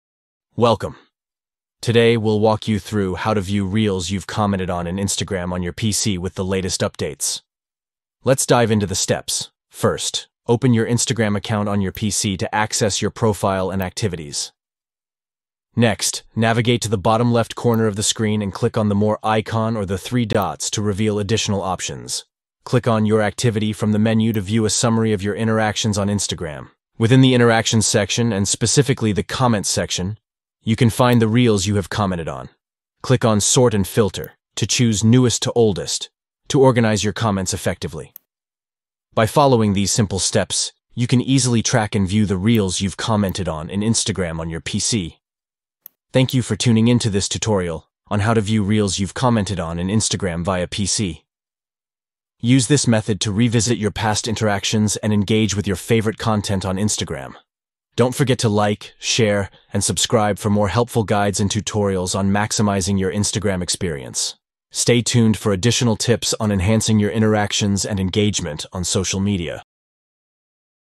How To View Reels You've Commented On In Instagram On PC (New Updated)
Learn how to view reels you've commented on in Instagram on PC with these steps:

1- Open your account
2- At the bottom left corner, click on the more icon or the three dots
3- Click on Your Activity
4- From Interactions section and Comments section, you can see reels you have Commented on, click on Sort and Filter to choose Newest to Oldest.

Discover the new updated method to easily find and organize your commented reels on Instagram. InstagramTips,# ReelsComments, SocialMedia.